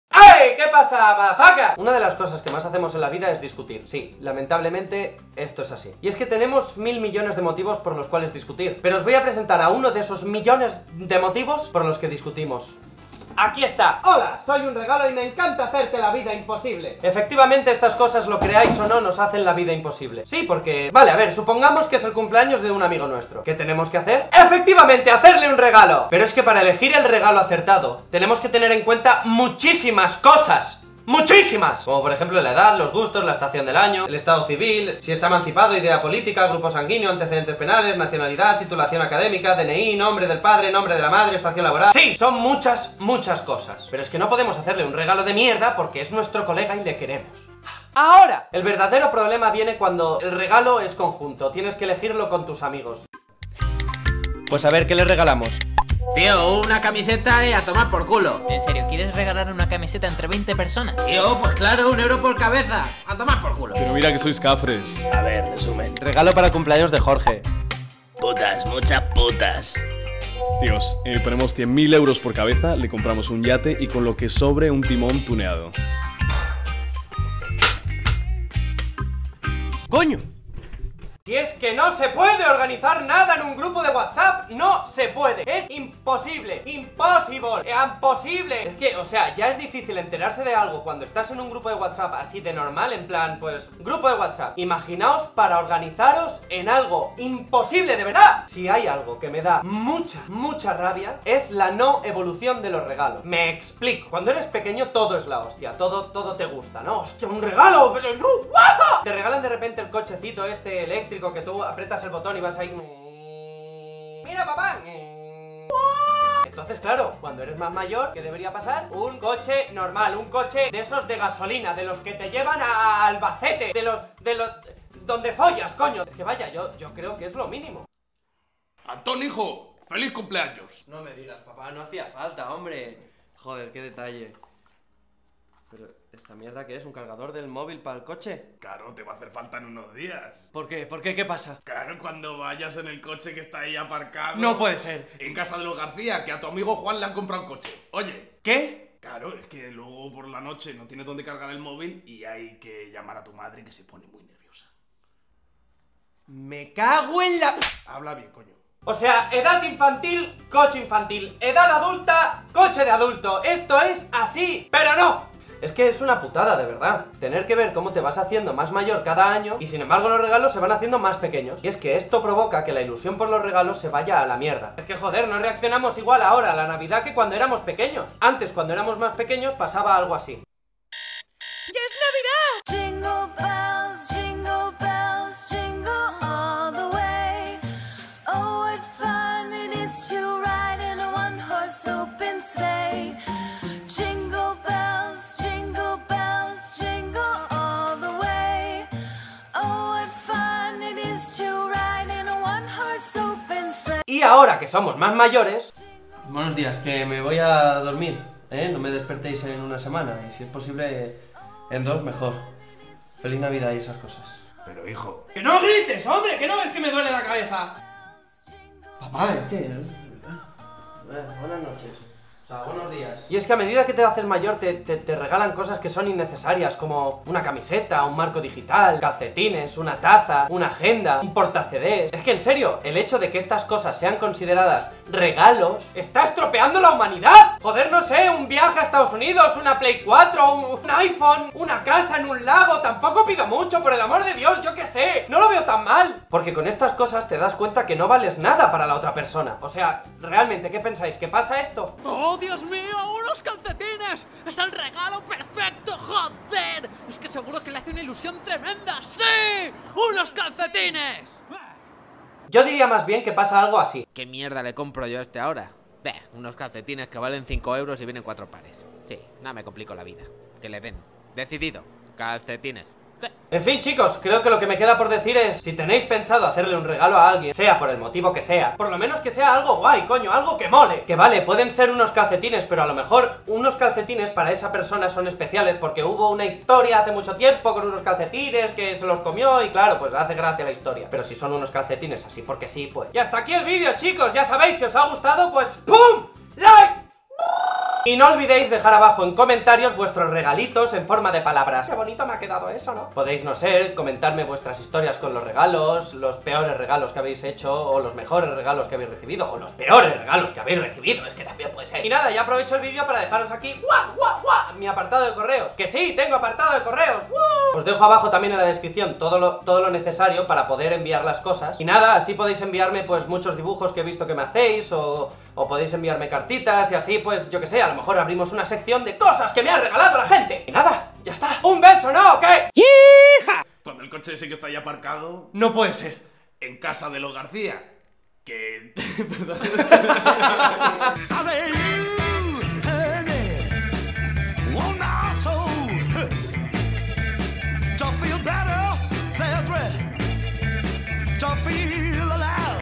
LOS REGALOS C/ WASABI HUMOR | Antón LoFer
Los regalos es algo que tenemos siempre presente en nuestras vidas! Pues aquí os traigo mi opinión sobre ellos!
Y gracias a Rubentonces por hacerme la parte final del video que mola un huevo coño seguirle que es el amo hace mucha gracia pero es una pesao xD
Quieres enviarme cositas?
Anton Lofer
Apartado de correos 28083
28020 MADRID

YEEHA!